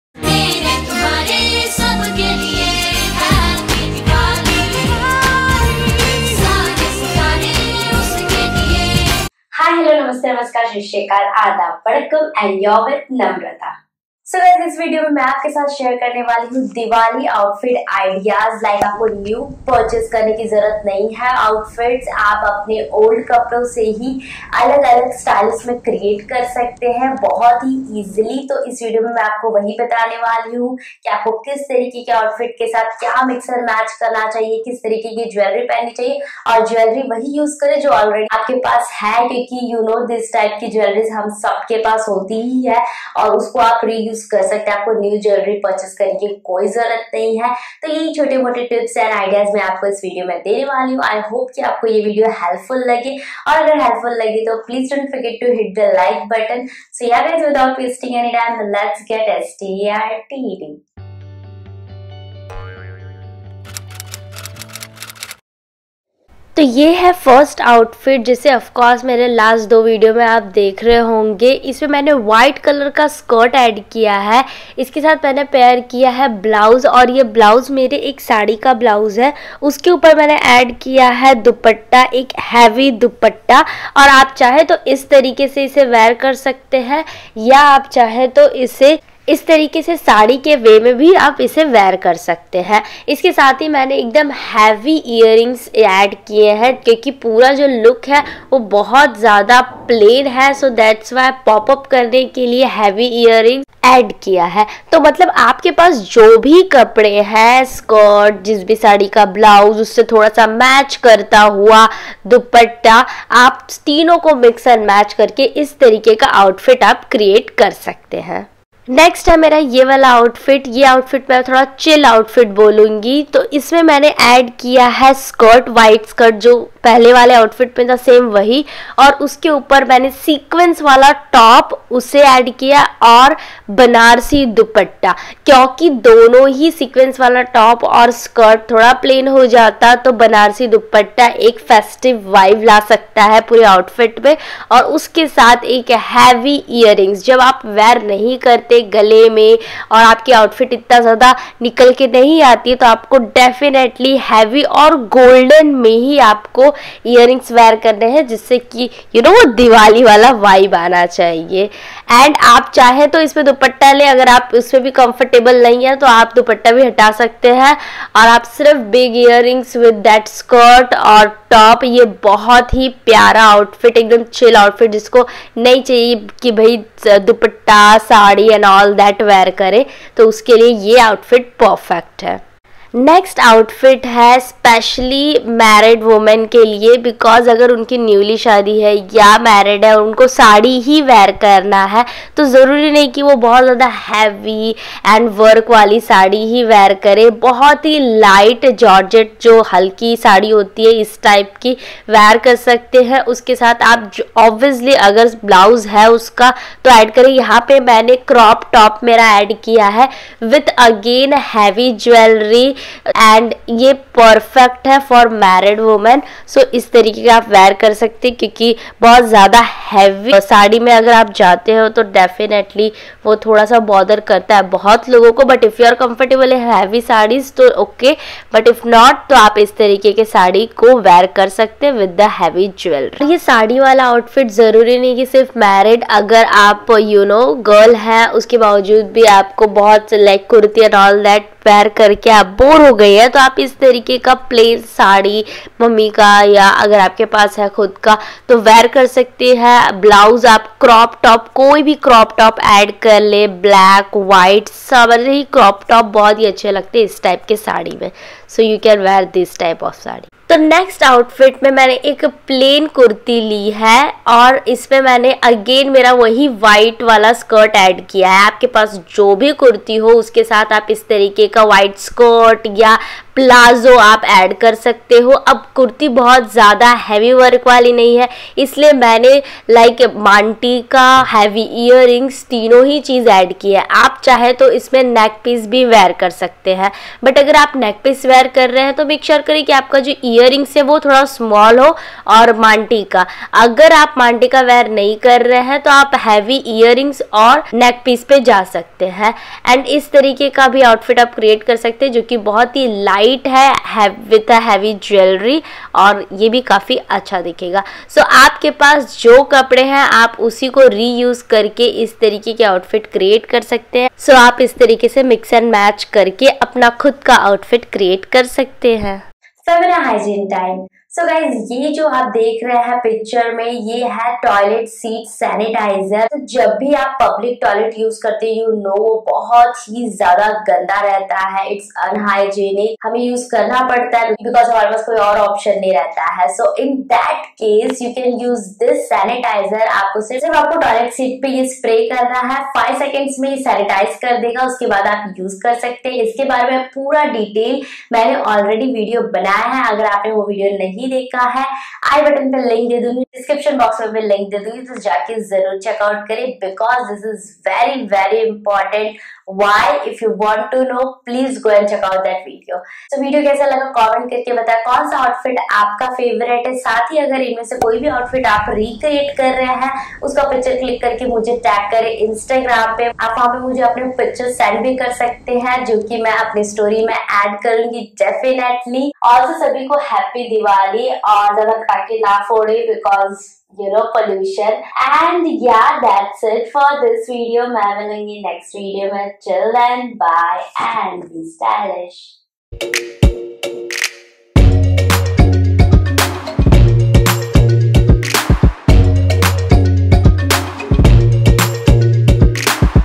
Diwali Outfit Ideas /Clothes Mix & Match Ideas/ Reuse your Old clothes this Diwali
Hey Guys! in this video, I am going to show you How to make reuse your old clothes.

 2) Which is BEST for WEDDING or PARTY+Maybelline FIT ME VS   COLORBAR Foundation  Review & Demo(IN HINDI)

Camera lens
Vlog Camera
Camera Stand